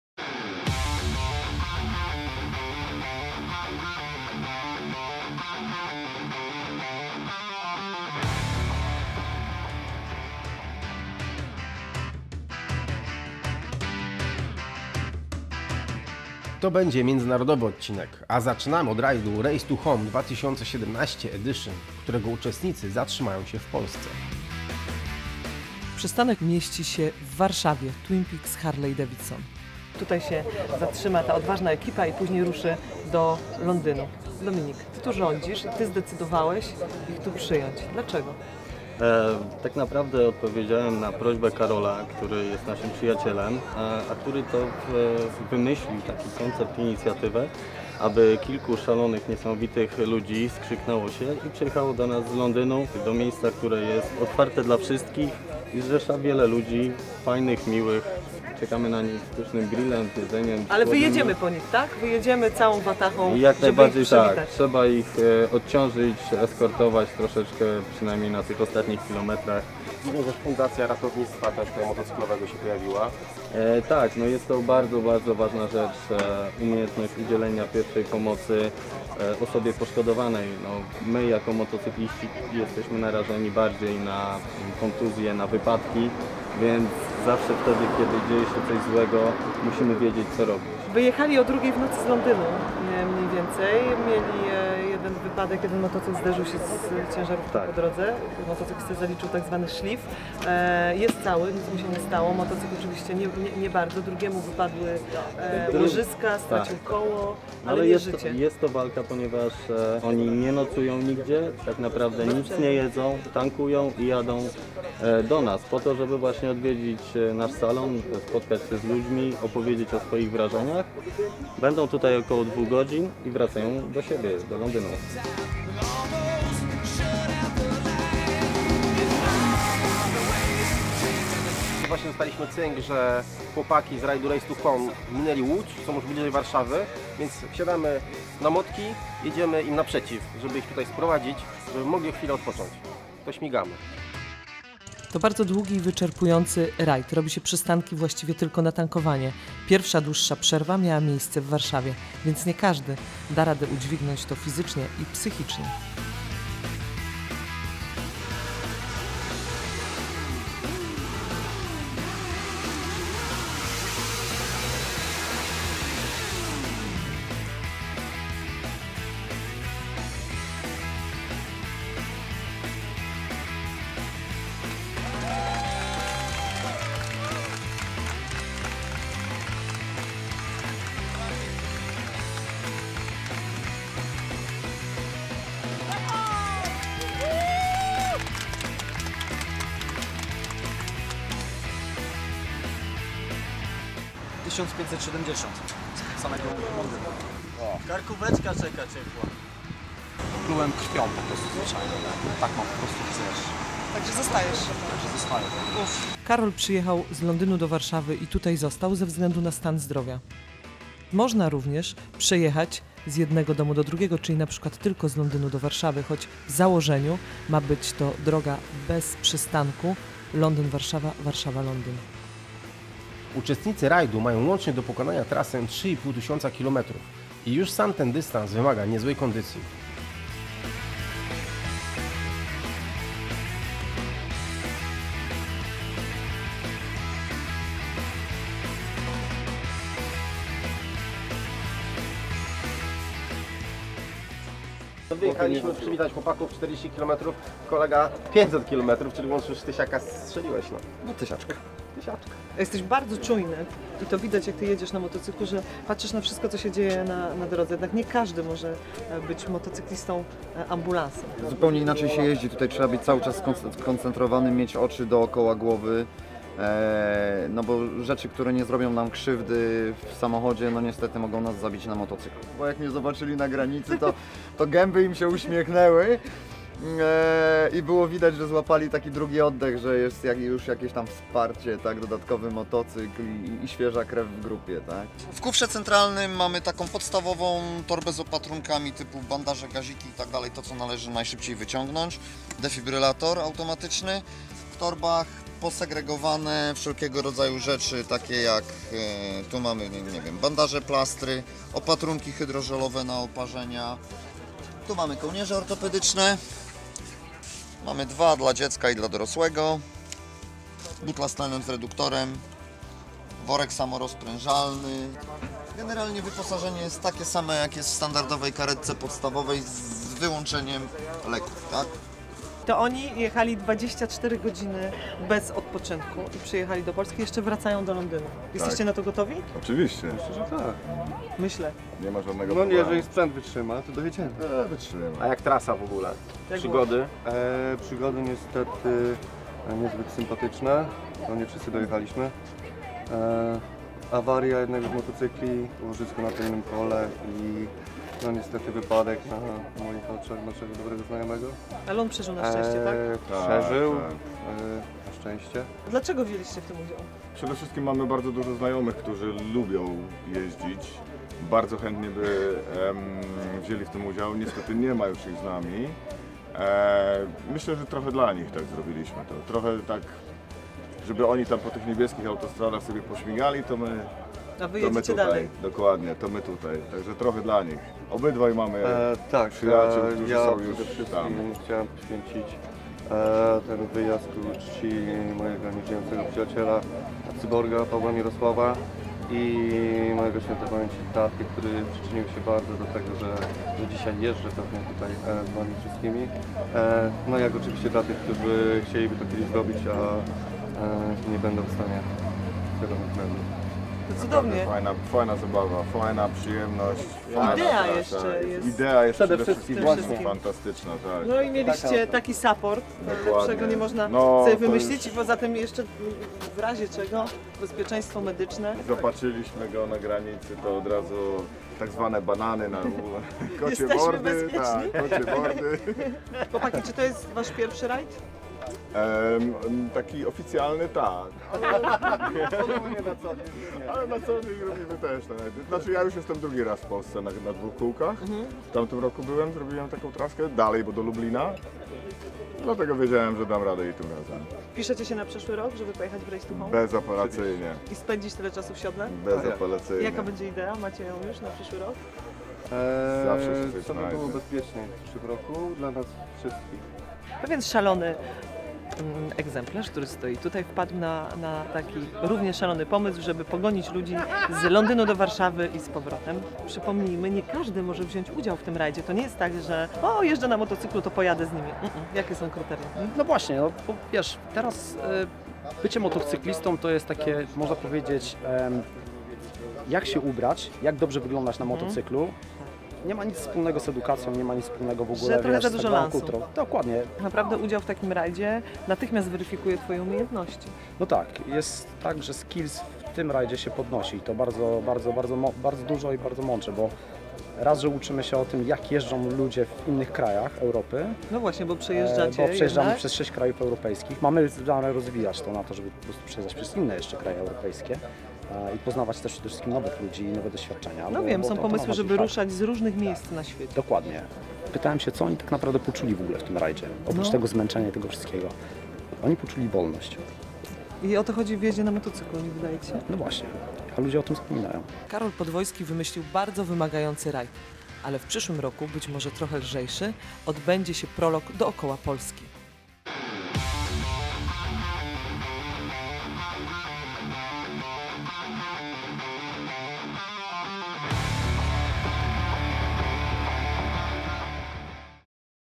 Race To Home 2017 - Rajd Londyn-Warszawa-Londyn
Ola i Robert witają w stolicy uczestników międzynarodowego rajdu Race to Home i rozmawiają o wrażeniach z przejazdu. Poznają też ekipę Fundacji Ratownictwo Motocyklowe Polska. Posłuchajcie co o rajdzie mówią jego uczestnicy i dlaczego wzięli w nim udział oraz jak należy udzielać pierwszej pomocy.